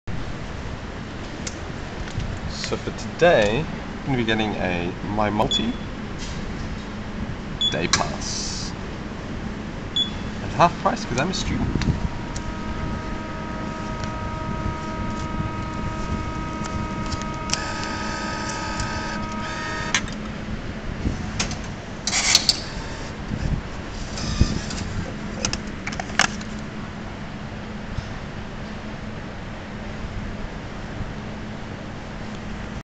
Buying a MyMulti Daily
Buying my ticket at the start of the day from the ticket machine at Bondi Junction station at 4:50AM.